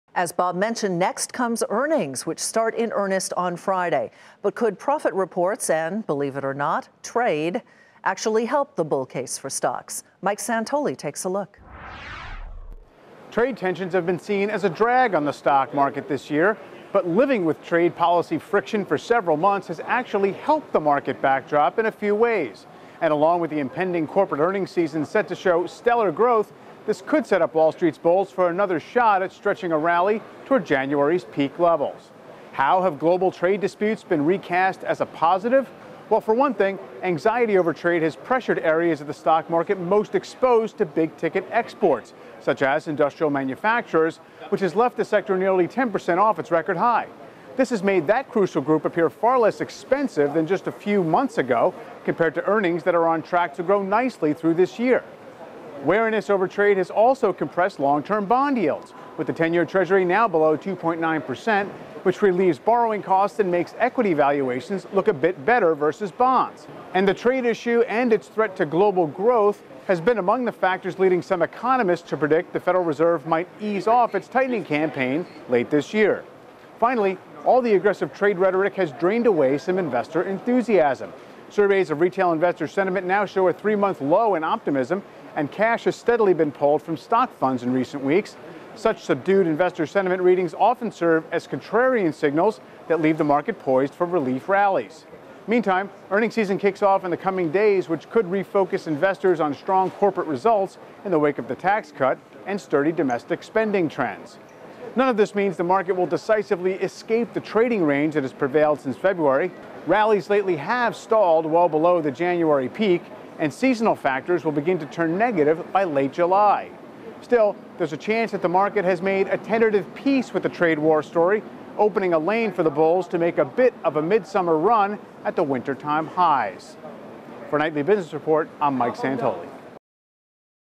Bulls look for signs of stock rally
Mike Santoli looks at whether the trade war and upcoming earnings reports can give markets another jolt of power.